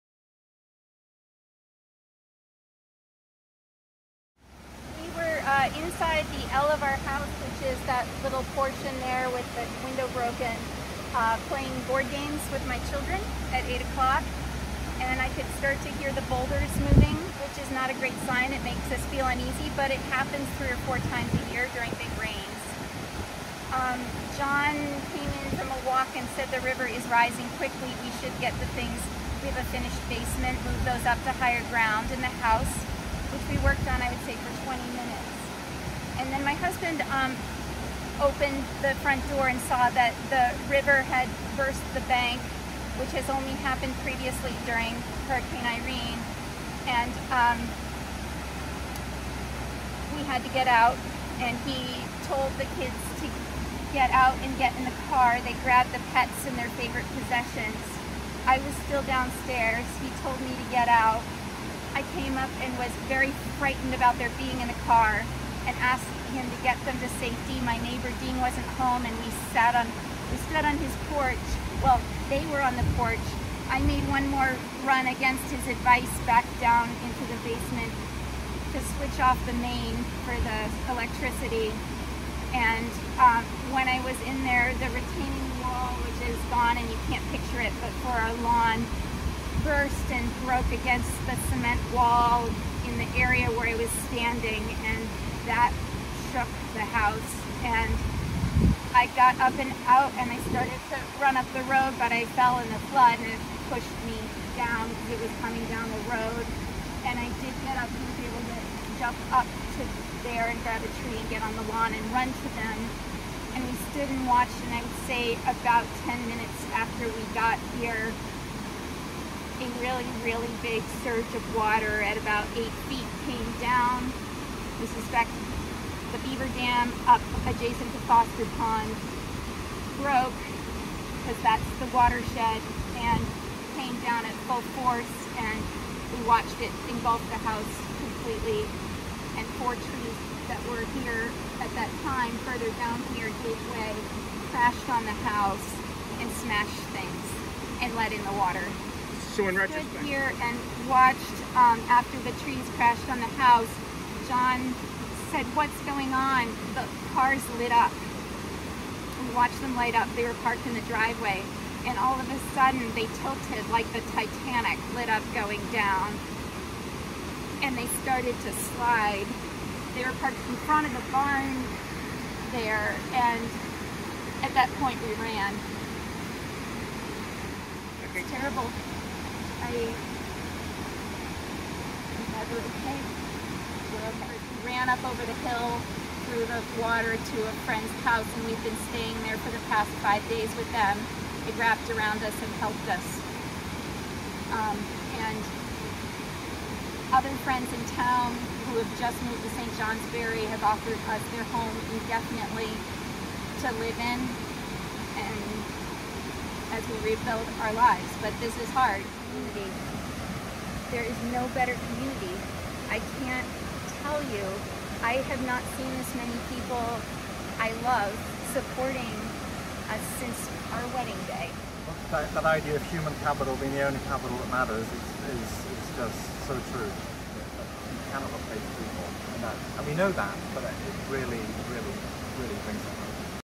Peacham couple recount escape from flash flood
Peacham, Vermont, residents John & Jenny Mackenzie tell the story of their July 10, 2024 escape from the flash flooding that destroyed their family's home on Gov Mattocks Road.